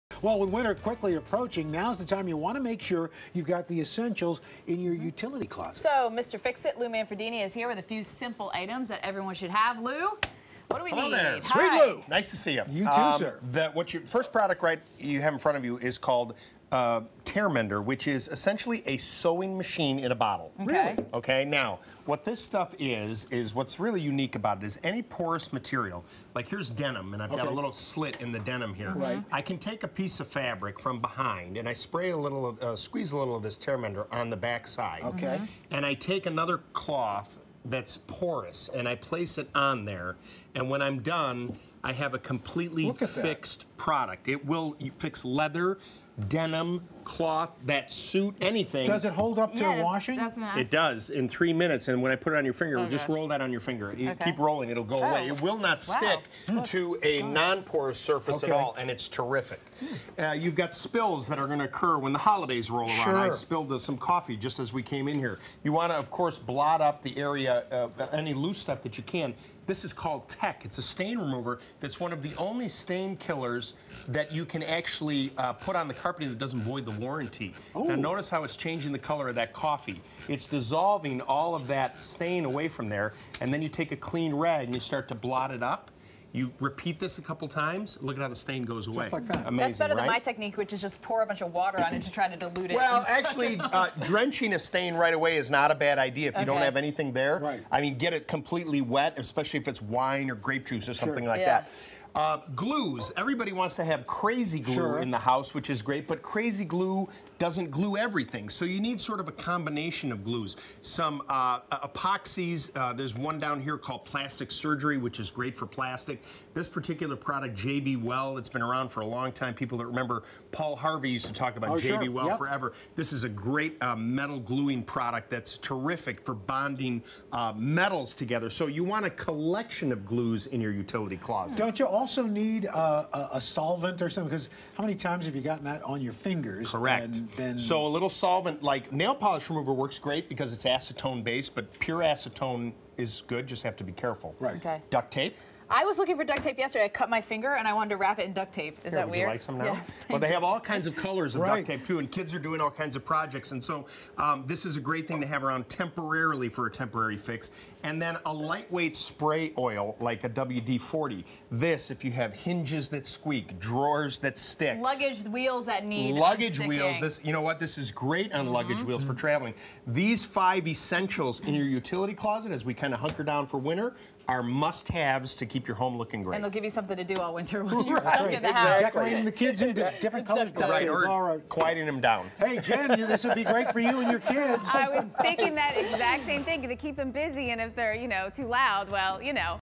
ICYMI: @loumanfredini shares 5 essentials to have in your utility closet
Mr. Fix-It, Lou Manfredini, shares with Al Roker and Stephanie Abrams 5 essentials items everyone should have in their utility closet.